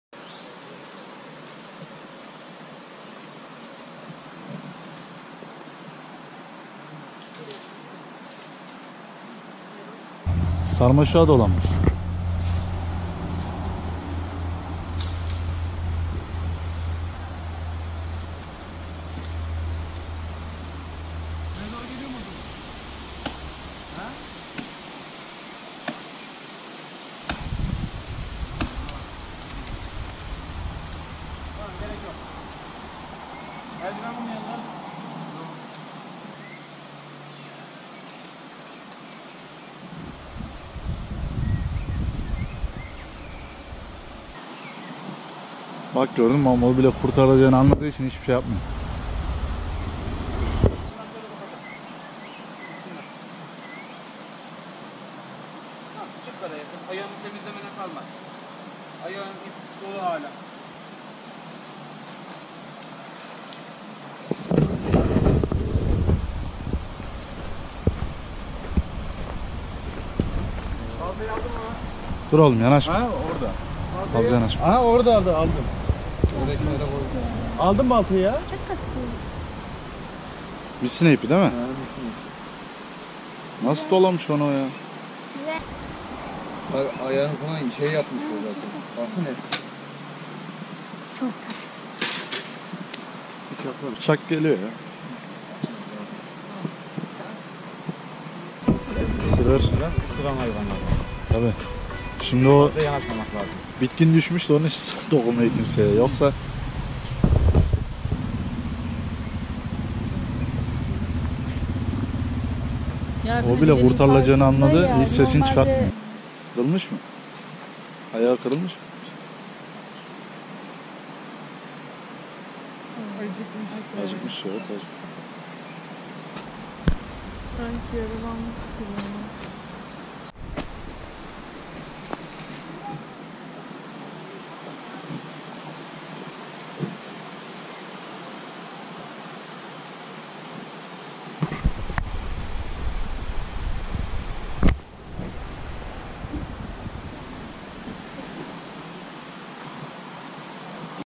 AYAĞINA İP DOLANAN KARGANIN YARDIMINA İTFAİYE EKİPLERİ YETİŞTİ
Karabük’ün Safranbolu ilçesinde ağaç dalında ayağına ip dolanan karganın yardımına itfaiye ekipleri yetişti.